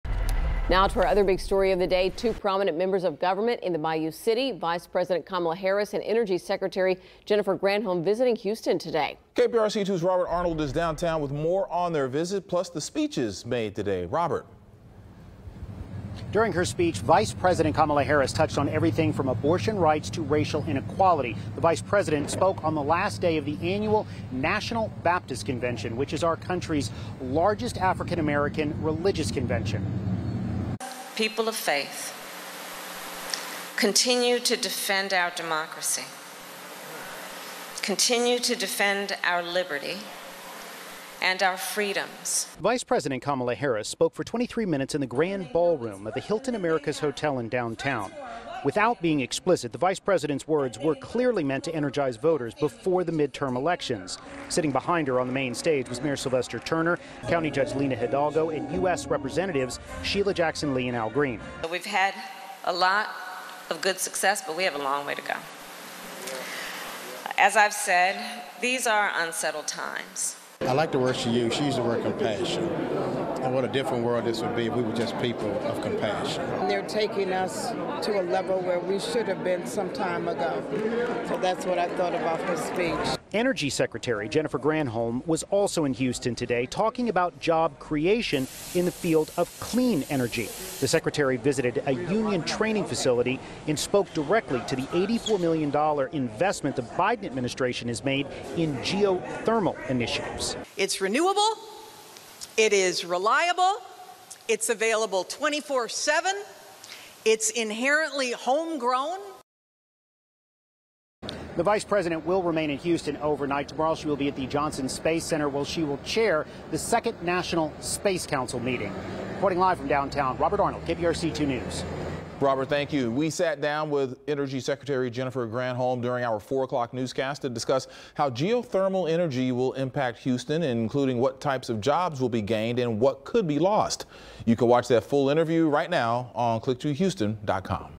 Vice President, energy secretary visit Houston
U.S. Secretary of Energy Jennifer M. Granholm visited Houston Thursday to discuss the potential of geothermal energy investments.